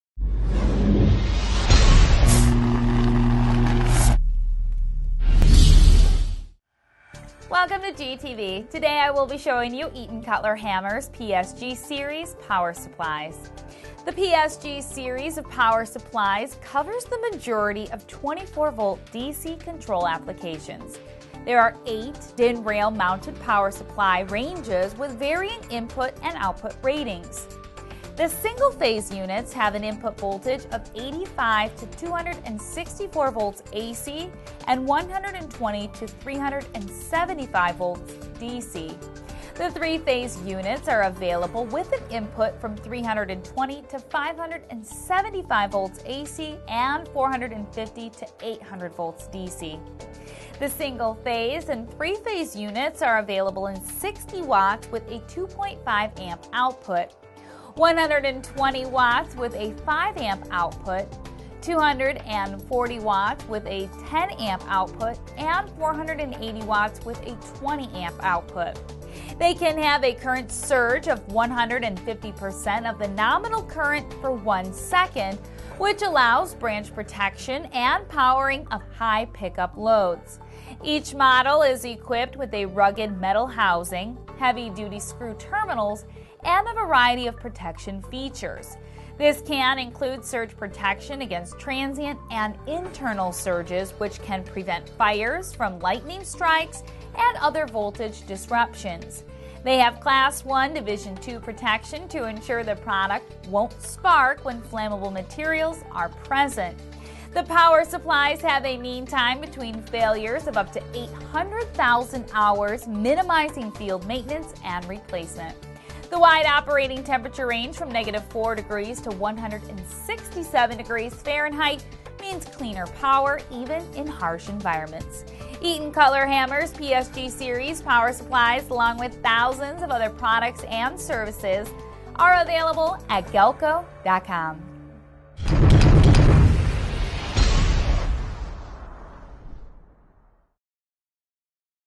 EATON Cutler-Hammer "PSG-Series" Power Supplies - A GalcoTV Overview
Eaton Cutler-Hammer PSG Series Power Supply presented by Katie Rydzewski for Galco TV.

Eaton's PSG Series of power supplies is designed to be a high-performance, high-quality line of products covering the majority of 24 Vdc control applications. With global certifications, compact size and an impressive operating temperature range, the PSG series fits a wide variety of applications at a competitive price.

The PSG family includes eight DIN-rail mounted power supplies with varying input and output ratings:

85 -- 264 Vac Input Models:
PSG60E — 2.5 amperes
PSG120E — 5 amperes
PSG240E — 10 amperes
PSG480E — 20 amperes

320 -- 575 Vac Input Models:
PSG60F — 2.5 amperes
PSG120F — 5 amperes
PSG240F — 10 amperes
PSG480F — 20 amperes